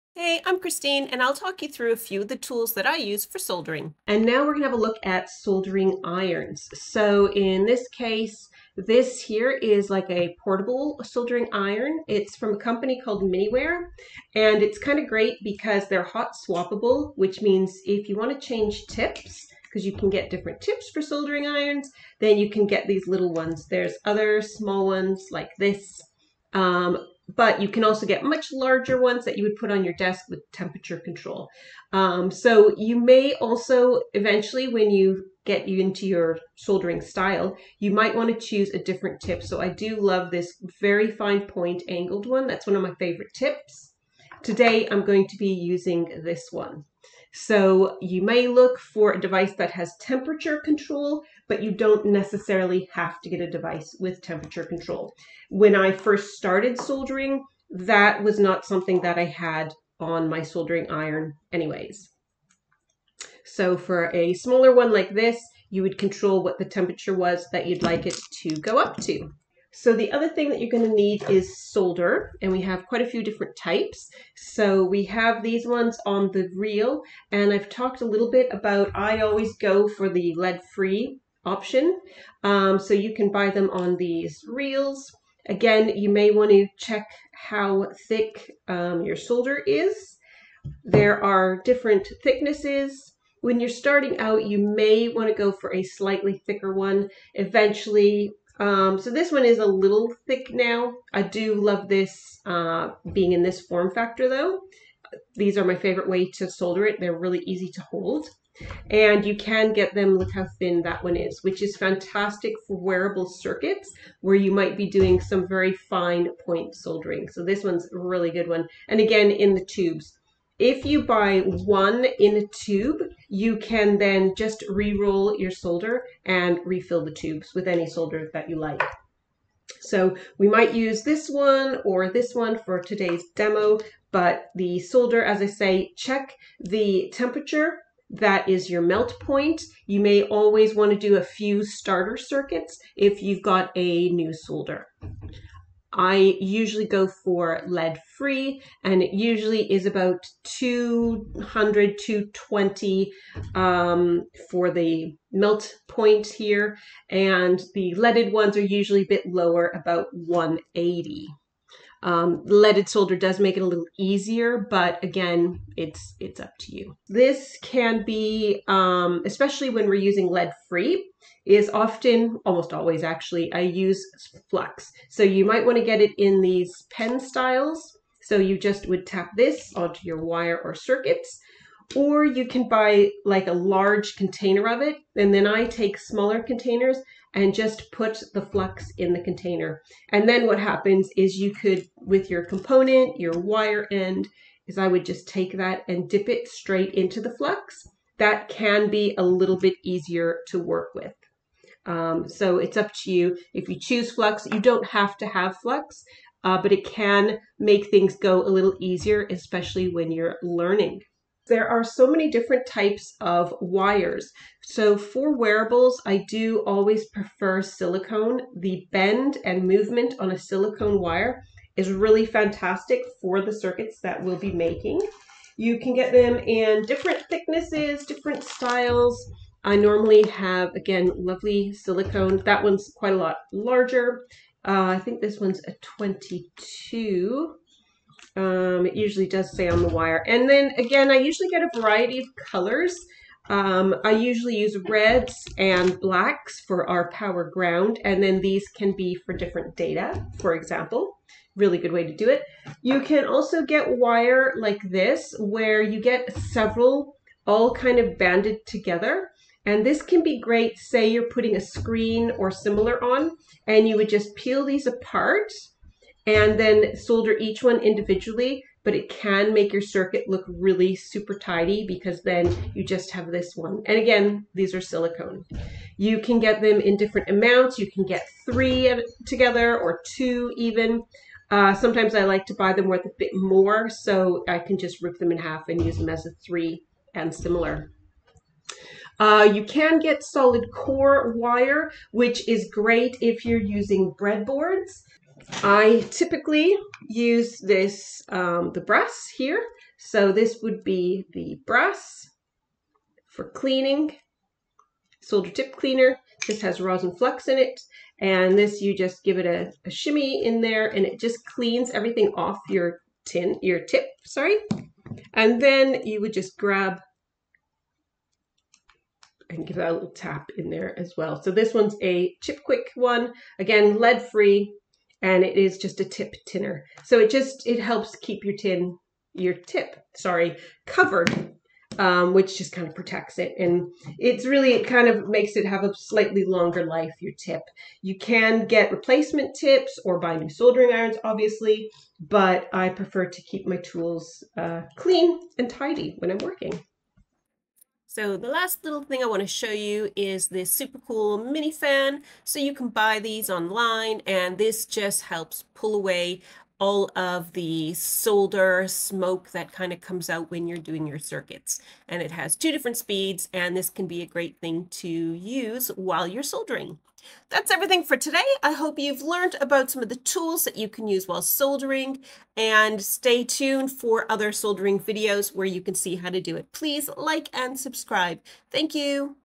The BEST Soldering Tools (and why you need them)
This is first of several videos for soldering basics. In this video I cover the basics of tools that are useful to have for soldering.

Learn to solder in under 5 minutes! 💜 This is a super quick start, and I've made additional videos to introduce you to the tools for soldering and some other fun soldering an ESP32 C3 board.

✔️ Discover innovative tools and materials for wearable prototyping—all in one place at Tinker Tailor! Dive into a world of creativity and convenience